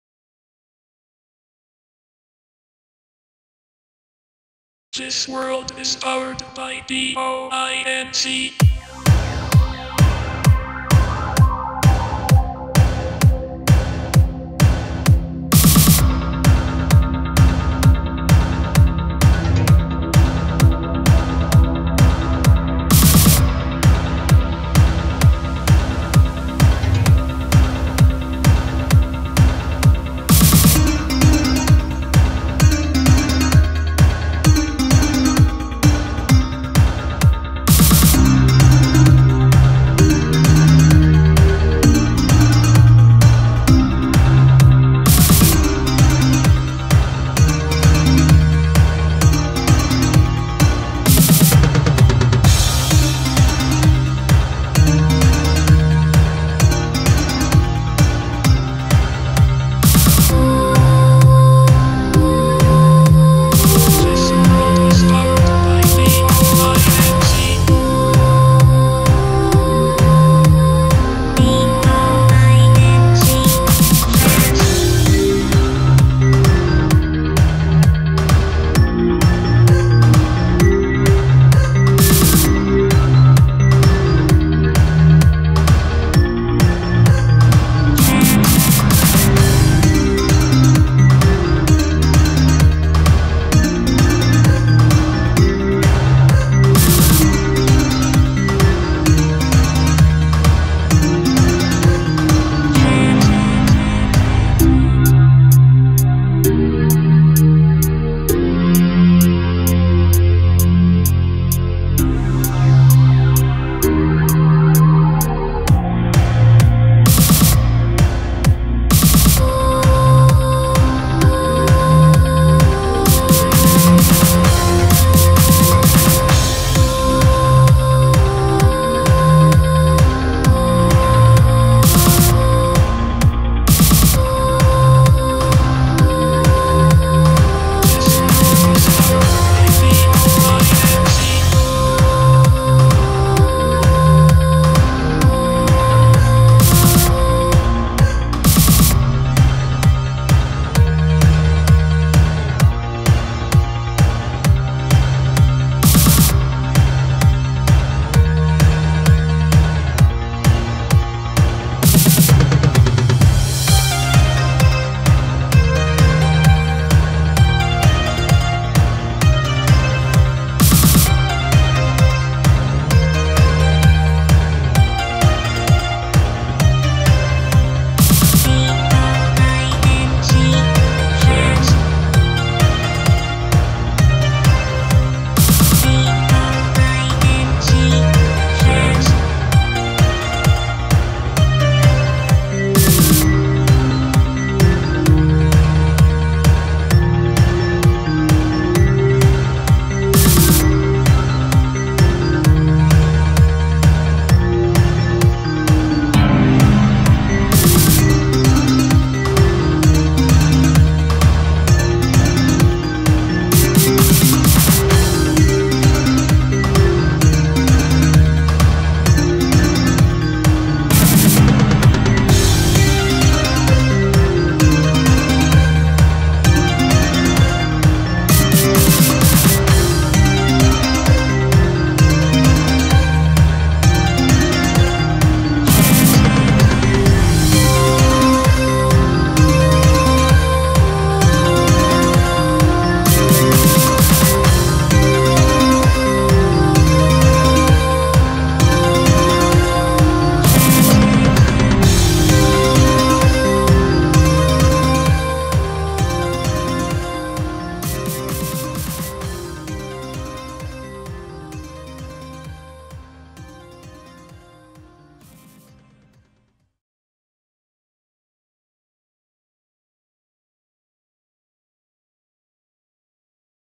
Powered by BOINC
This is second version of my video clip titled "Powered by BOINC". This time it contains valuable information about BOINC project from World Community Grid Web page. I hope you will enjoy it. You can use link below if you want join to World Community Grid: